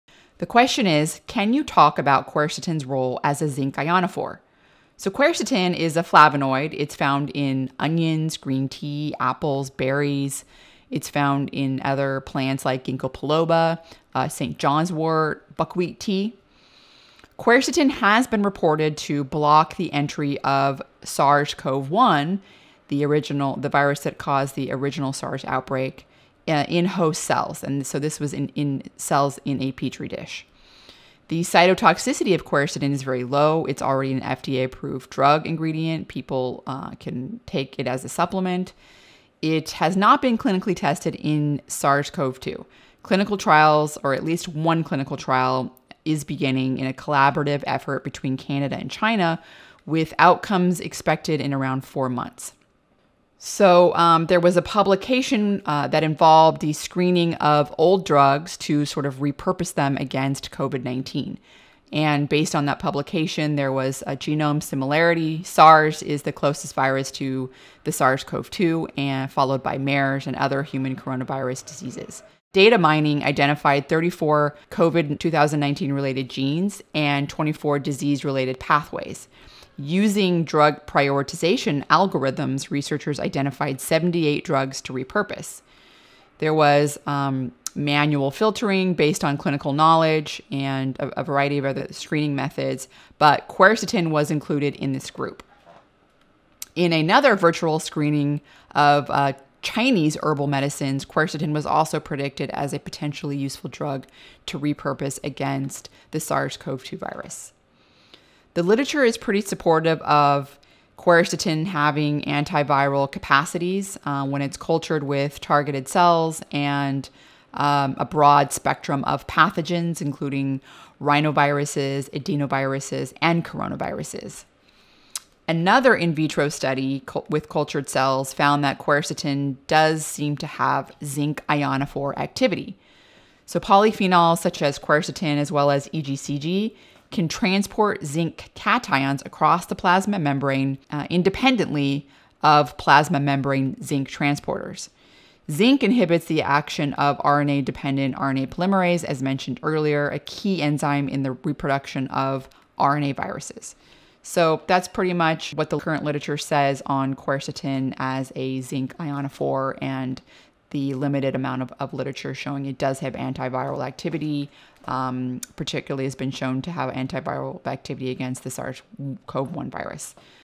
Quercetin is a zinc ionophore with antiviral activity | Rhonda Patrick
Quercetin is a flavonoid found in capers, peppers, and onions. It has been identified as a zinc ionophore which is a compound that can transport zinc ions across a cell membrane. In this clip, Dr. Rhonda Patrick discusses quercetin's identified antiviral activity against SARS-CoV-1 and its role in zinc transport.